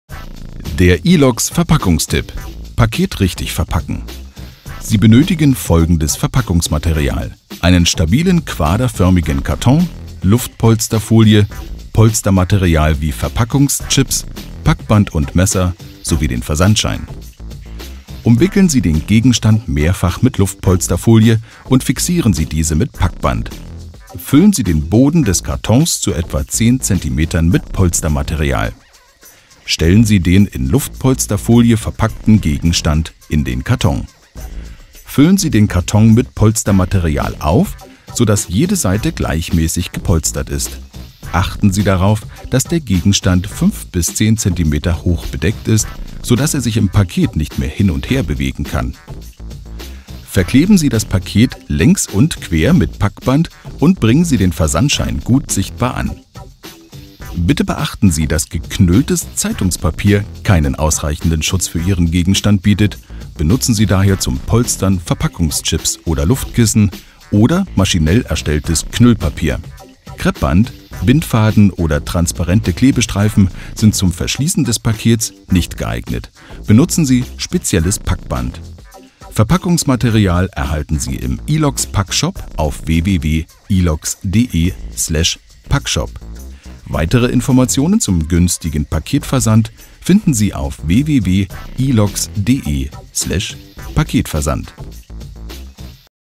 Verpackungstipps | Pakete sicher verpacken
Der iloxx Verpackungstipp: Paket sicher verpacken

Mit dem iloxx Paketversand versenden Sie Pakete bequem mit Abholung und der Zustelloption Predict.

Für den Versand von Paketen benötigen Sie folgendes Verpackungsmaterial:
- Stabilen quaderförmigen Karton
- Luftpolsterfolie
- Polstermaterial, z.B. Styroporchips
- Klebeband
- Schneidemesser
- Versandschein

So verpacken Sie Pakete sicher:
- Gegenstand mehrfach mit Luftpolsterfolie umwickeln und mit Klebeband fixieren
- Boden des Karton, etwa 10 cm hoch, mit Polstermaterial befüllen
- Verpackten Gegenstand in den Karton stellen / legen
- Karton mit Polstermaterial auffüllen
- Gegenstand sollte 5 bis 10 cm hoch bedeckt sein
- Karton mit Klebeband verschließen
- Versandschein gut sichtbar, auf der größten Seite des Pakets, anbringen

Bitte beachten Sie, dass geknülltes Zeitungspapier keinen ausreichenden Schutz für Ihren Gegenstand bietet und sich Bindfaden oder transparente Klebestreifen nicht zum Verschließen des Pakets eignen.